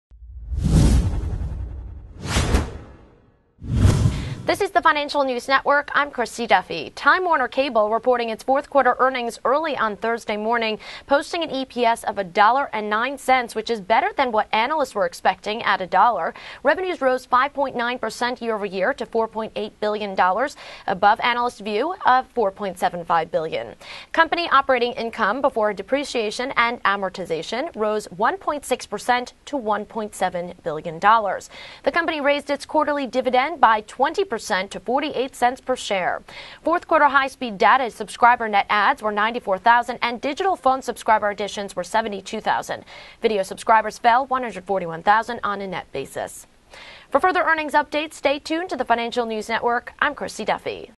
Time Warner Cable Tops Q4 EPS and Sales Estimates, Raises Dividend
Time Warner Cable (NYSE:TWC) reported Q4 EPS of $1.09, better than the consensus estimate of $1.00. Revenue rose 5.9% year-over-year to $4.80 billion, above analyst estimates of $4.75 billion. Company OIBDA rose 1.6% to $1.7 billion.

The company raised its quarterly dividend by 20% to 48 cents per share.

Q4 high-speed data subscriber net adds were 94,000 and Digital Phone subscriber additions were 72,000.